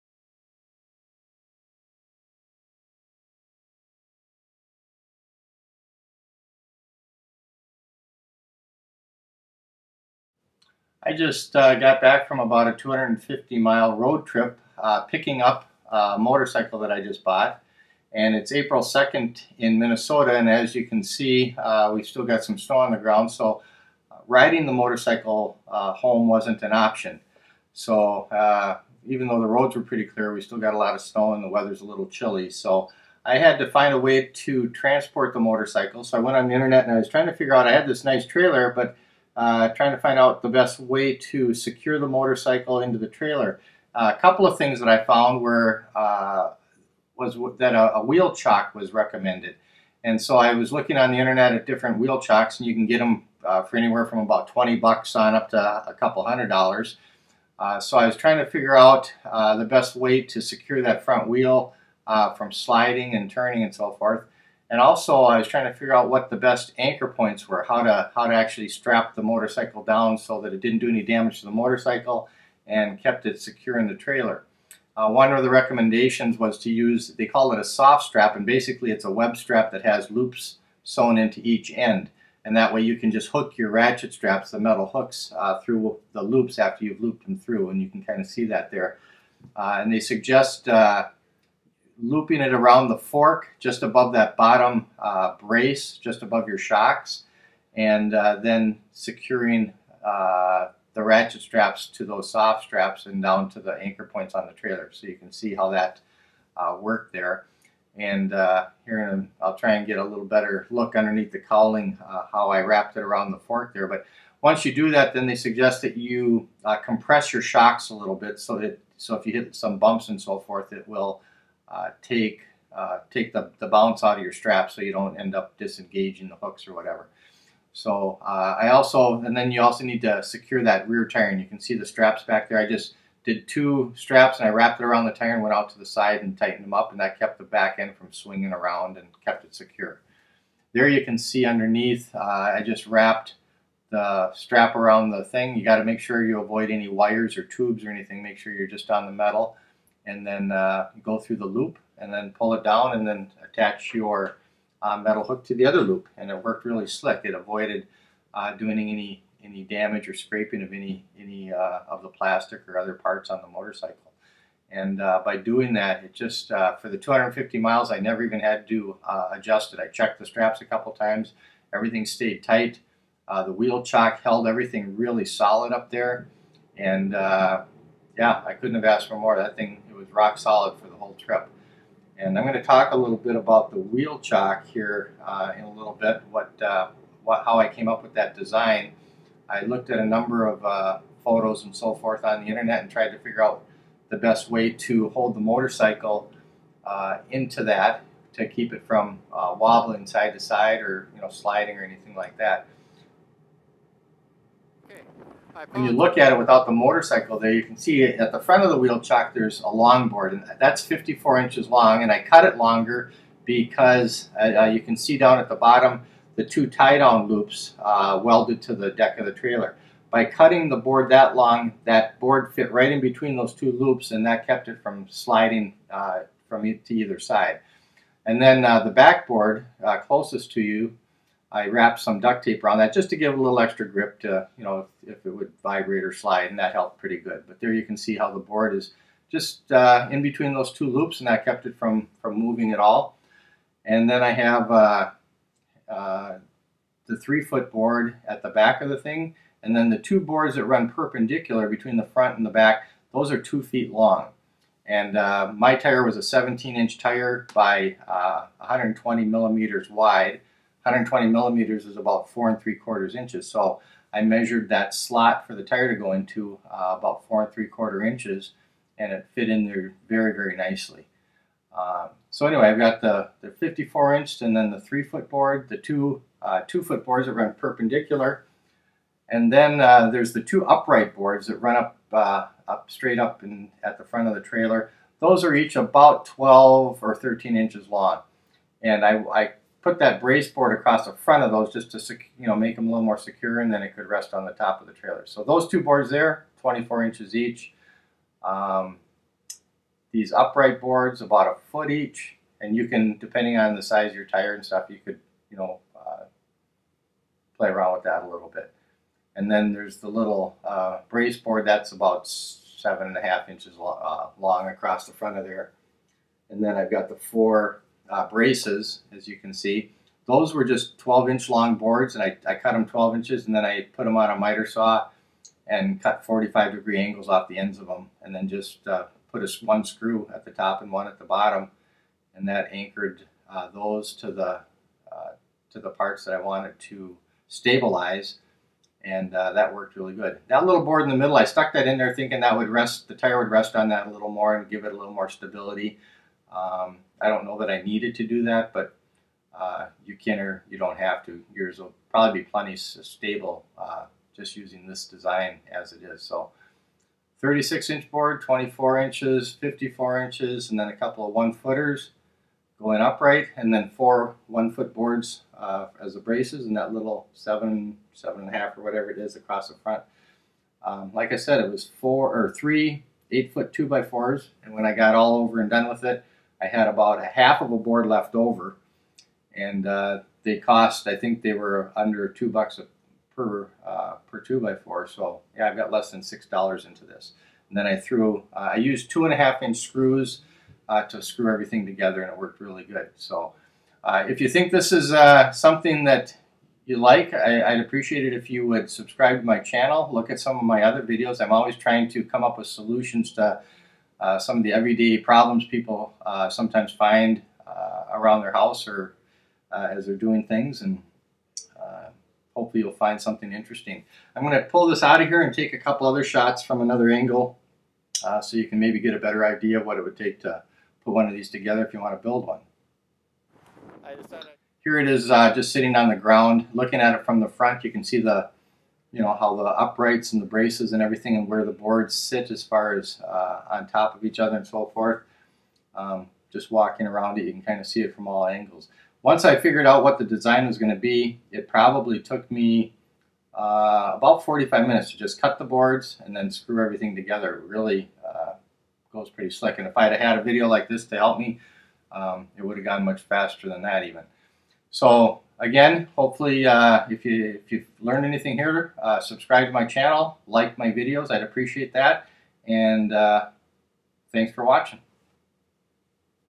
How to Build A Motorcycle Wheel Chock & Transport your Bike Securely
Build a wheel chock for your motorcycle for a fraction of what you would pay at the store! This video shows how to make a chock, and how to best secure your bike for transport on a trailer. Total cost...under $6.00.